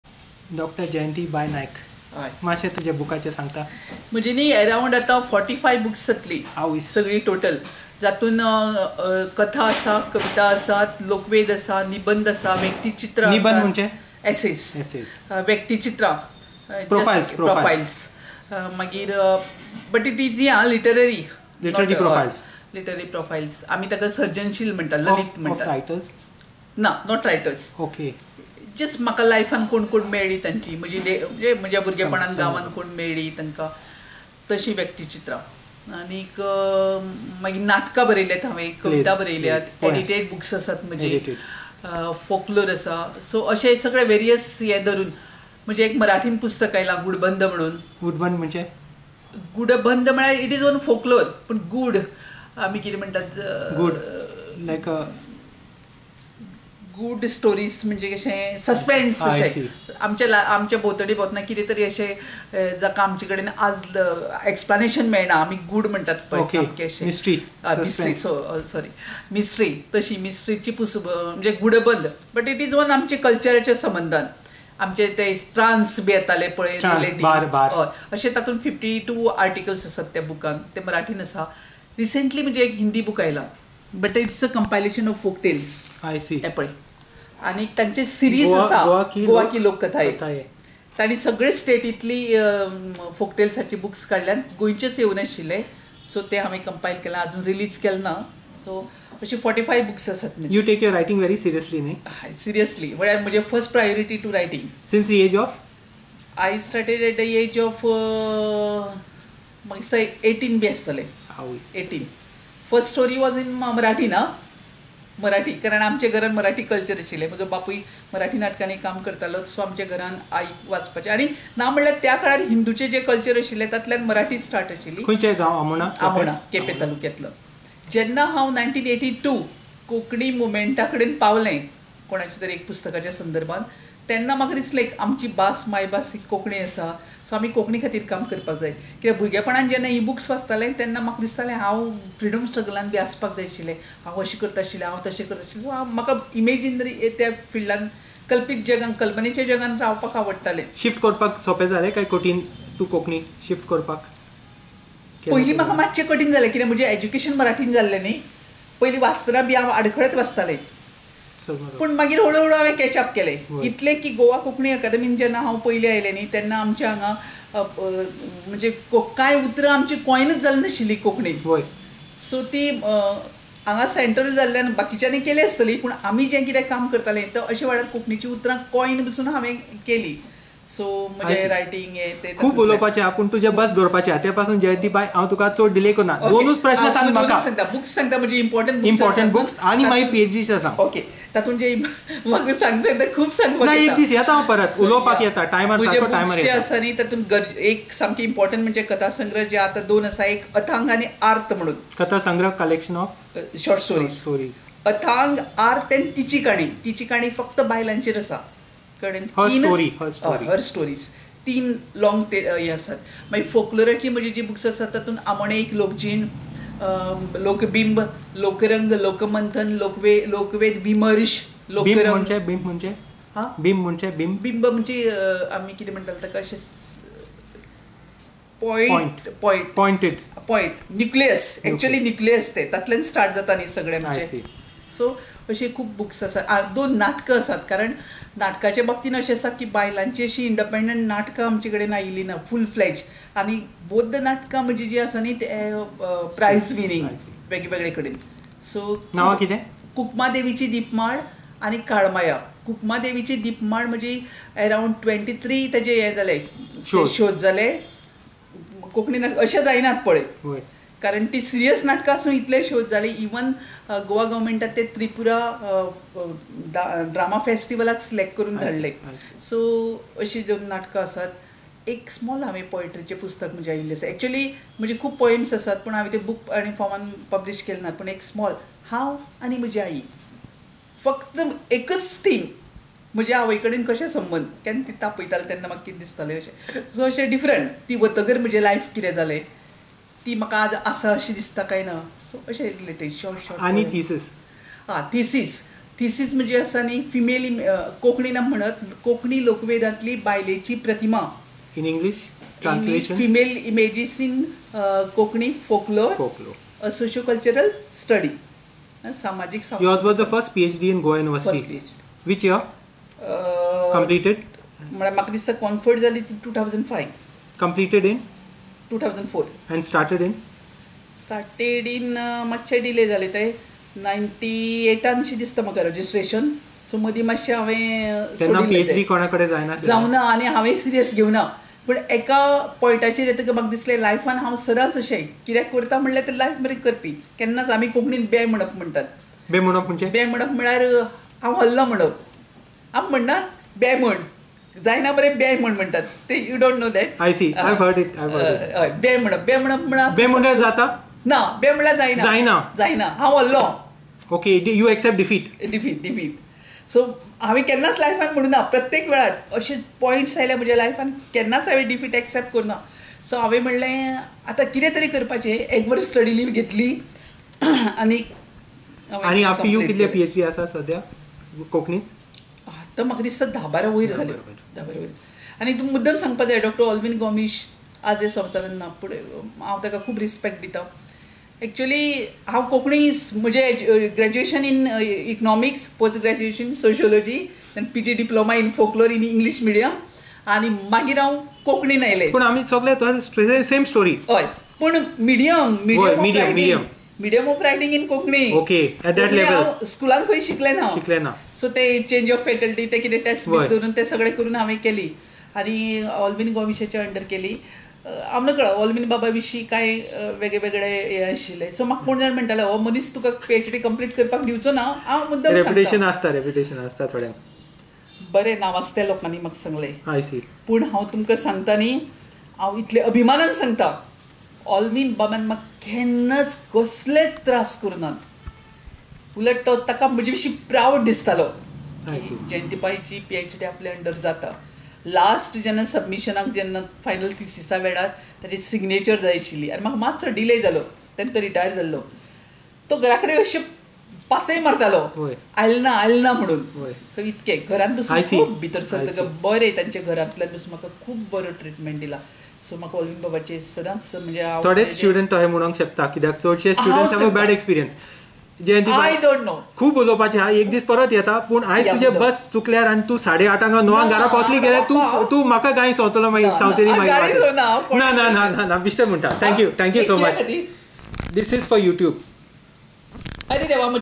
Dr Jayanti Naik, the first PhD from Goa University's Konkani Department, on her writing and books
Late on a weekday evening, I reached Dr Naik's office on some other errand. She was just finishing for the day, and I (mis)used my earlier connections with her to squeeze out a quick interview about her books, her approach towards writing, and her thesis. She wrote the first thesis in the Konkani Department of the then-fledging Goa University. The haste on my part here reflects the late hour of the evening, at which time she had to catch the bus back to distant Amona, her village in Quepem taluka, perhaps 45 or so km and two to three buses  away -- quite a distance by Goa standards. Interview by Frederick Noronha/FN